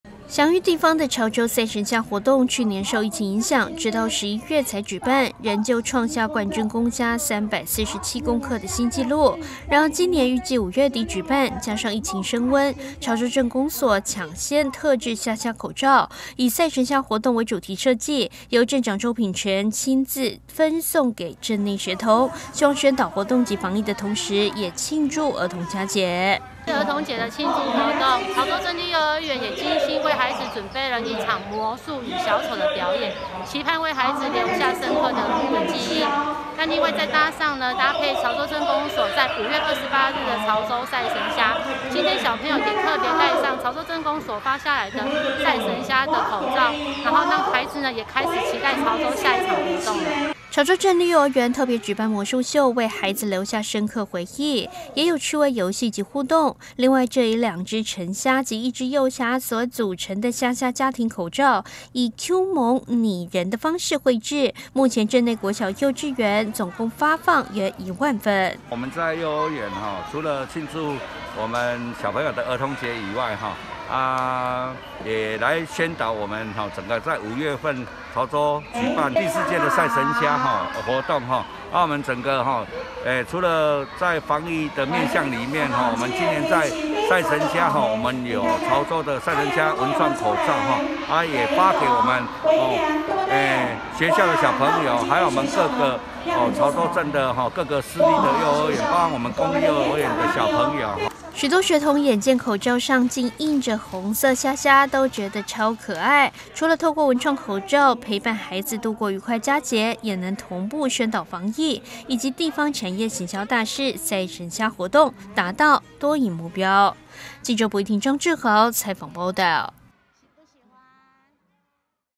潮州鎮/產業觀光 111 0404 潮州鎮公所特製「蝦蝦」防疫口罩 分送鎮內學童
潮州鎮公所每年舉辦「賽神蝦」活動推廣鎮內養蝦產業，今年更特製1萬份的「賽神蝦」口罩，除了宣傳賽神蝦活動，也因應近期疫情升溫，做防疫宣導，另外可愛的蝦蝦口罩還有親子版，因此也希望作為送給小朋友的兒童節禮物。